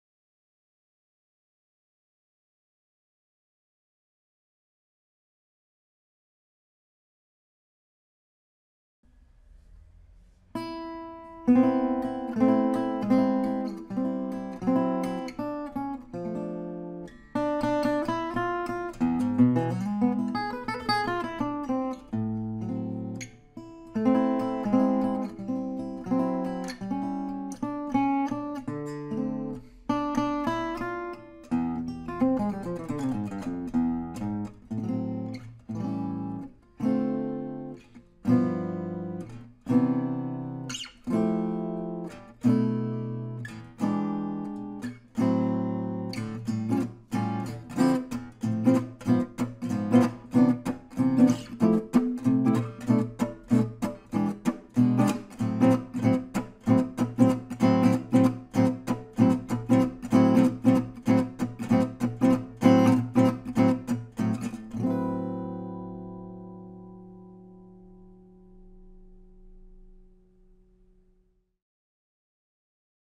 J.Castelluccia Luthier - Guitare Jazz Modèle 65TH bd
Démo vidéo du modèle 65th BD avec le guitariste Benjamin Blackstone. (Canon EOS 600D)

Video of demonstration of the 65th model D hole.
Performed with the participation of the guitarist Benjamin Blackstone.